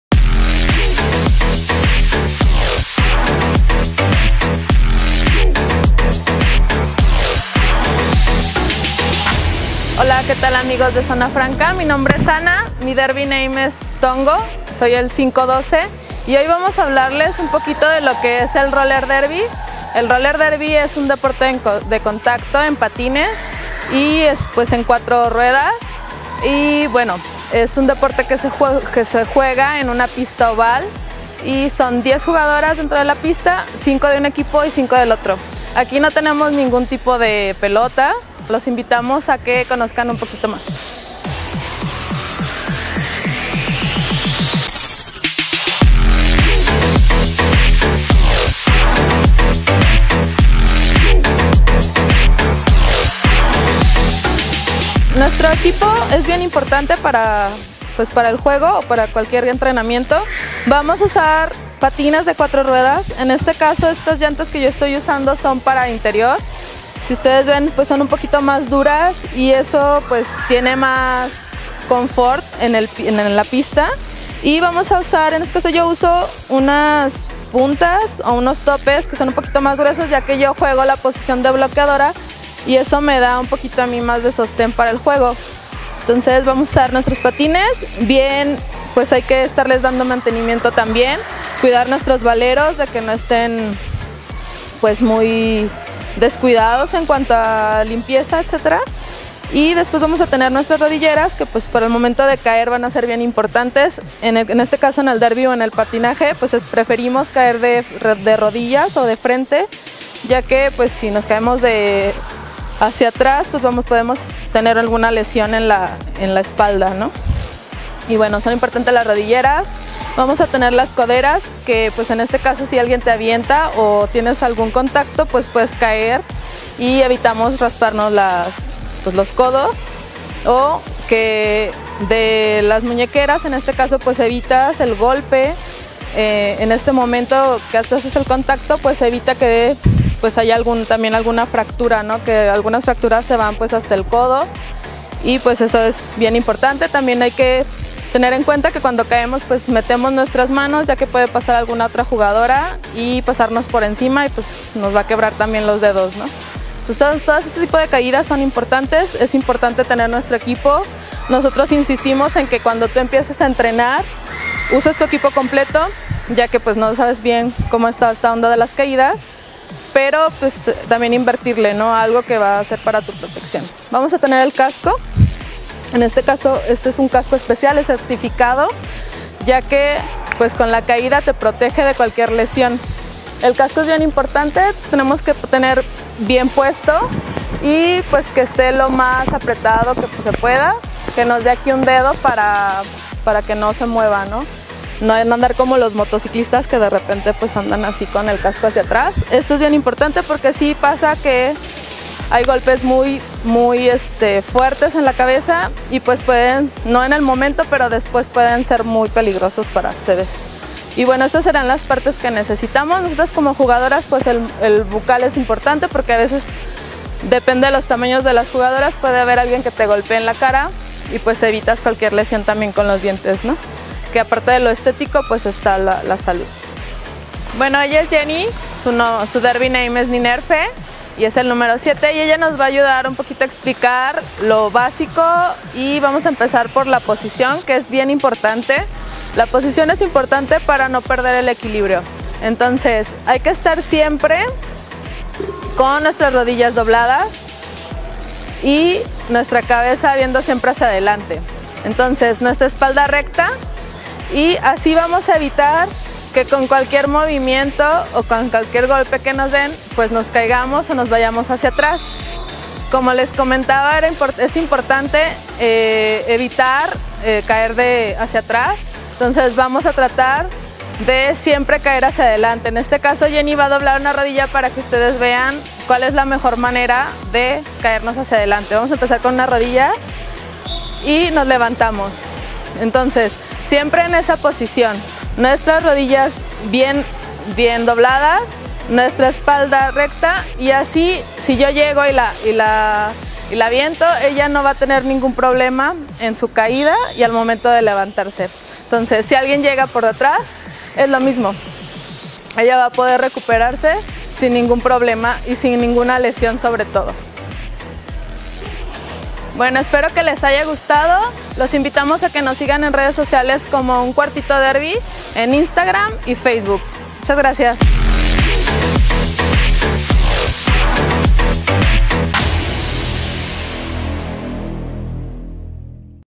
Conoce el Roller Derby | Cultura Física
Aprende las nociones básicas del Roller Derby, con las integrantes de "Un Cuartito de Noticias Derby".